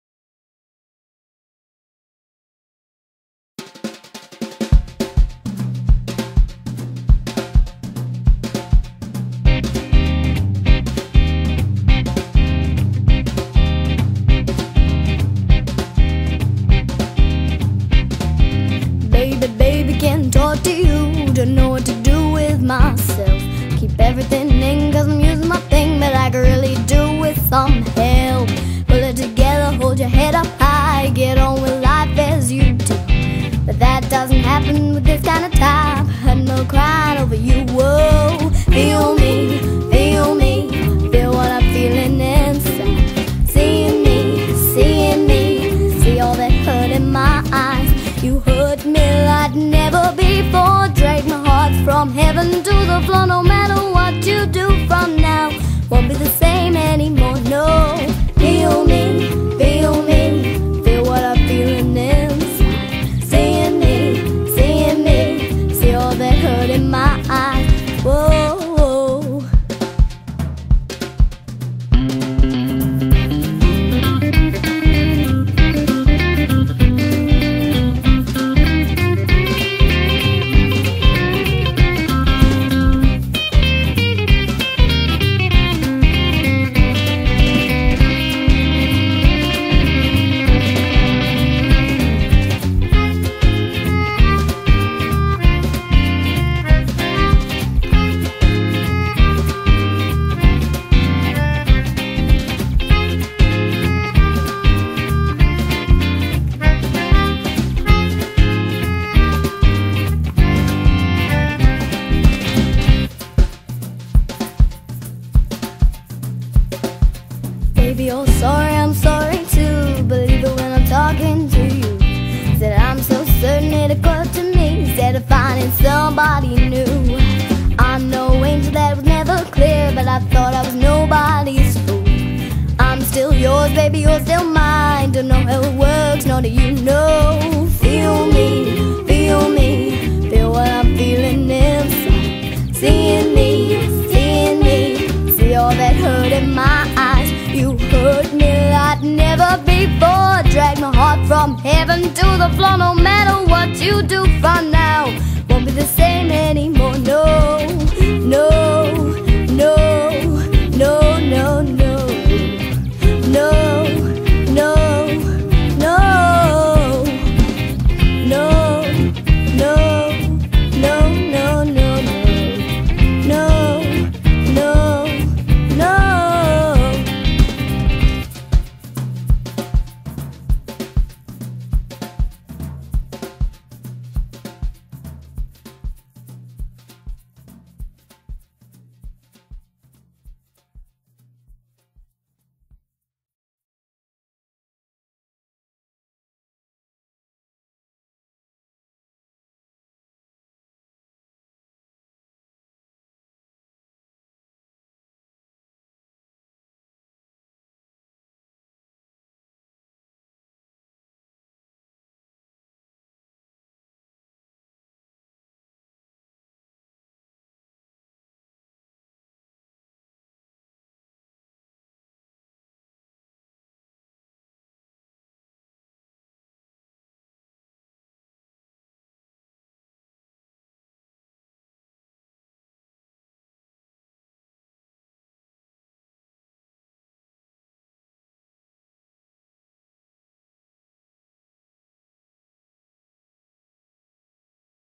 Battlestar Melodica-was All Day Breakfast featuring Anna
Our mates band. Anna one of the band members Daughter is singing one of their covers. She is 10 years old!!Absolutely amazing!